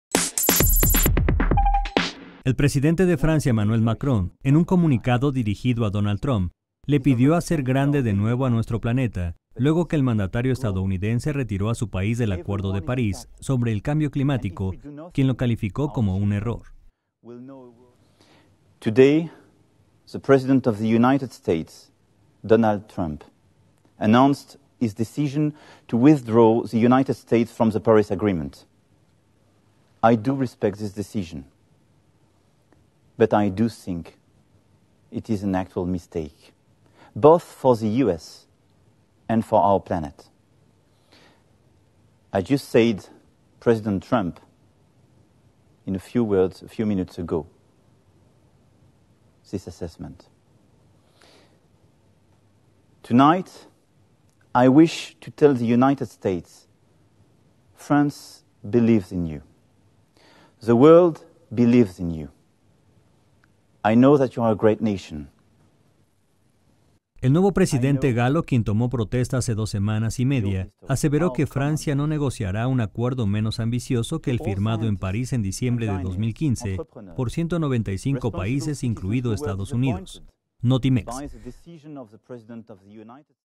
Macron pide a Trump "hacer grande de nuevo al planeta"
El presidente de Francia, Emmanuel Macron, habló hoy en inglés a Donald Trump y le pidió "hacer grande de nuevo a nuestro planeta", luego que el mandatario estadunidense retiró a su país del Acuerdo de París sobre cambio climático.